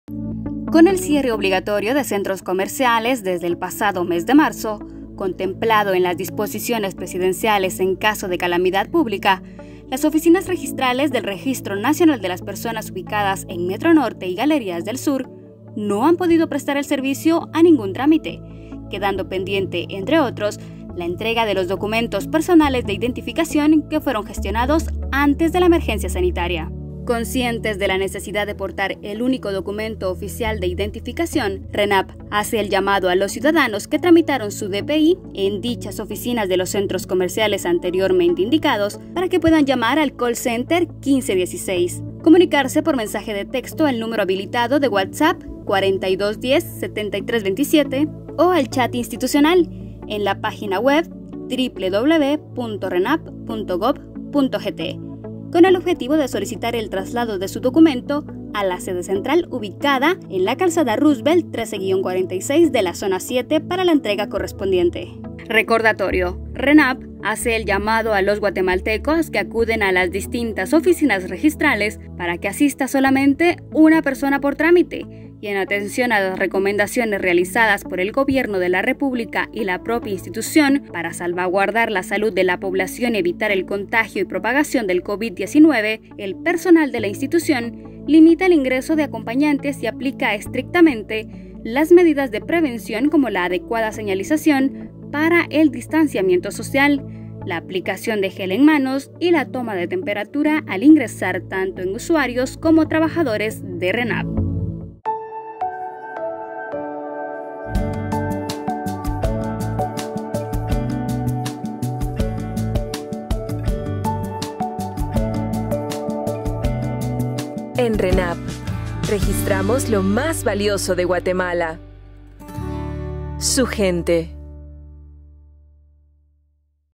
OFICINAS DE RENAP EN GALERÍAS DEL SUR Y METRONORTE
Comunícate con RENAP y realiza el traslado de tu Documento Personal de Identificación -DPI- a la sede central, ubicada en la Calzada Roosevelt.